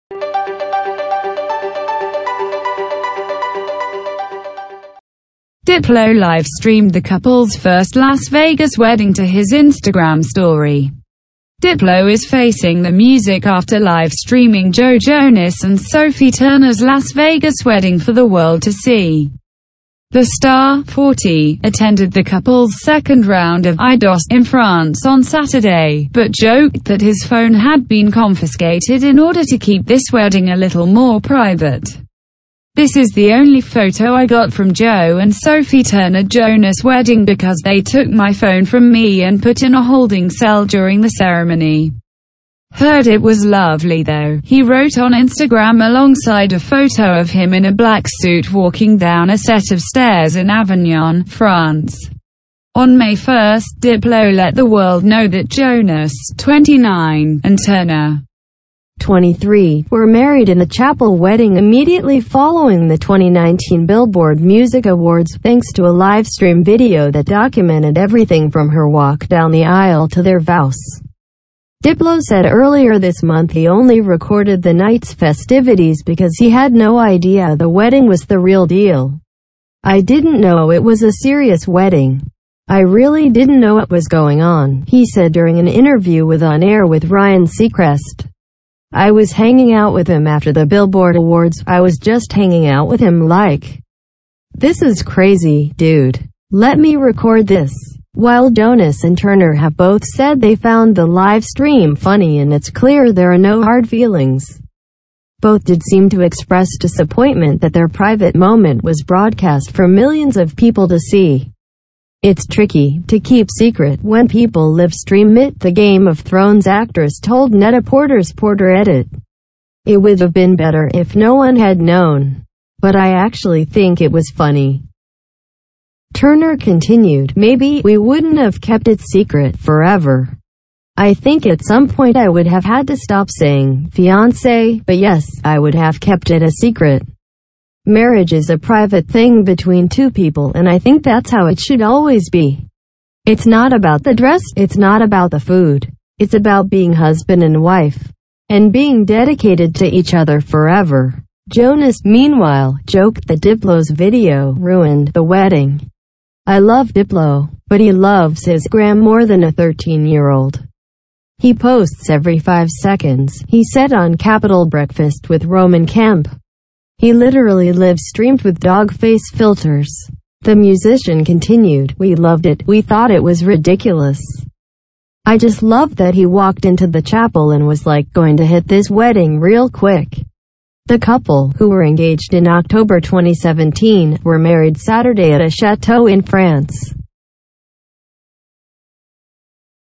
Diplo Jokes Joe Jonas & Sophie Turner Took His Phone for Second Wedding After He Filmed Their First
Diplo is facing the music after live streaming Joe Jonas and Sophie Turner’s Las Vegas wedding for the world to see.

The star, 40, attended the couple’s second round of “I dos” in France on Saturday, but joked that his phone had been confiscated in order to keep this wedding a little more private.

“This is the only photo I got from Joe and Sophie Turner Jonas wedding because they took my phone from me and put in a holding cell during the ceremony. Heard it was lovely tho,” he wrote on Instagram alongside a photo of him in a black suit walking down a set of stairs in Avignon, France.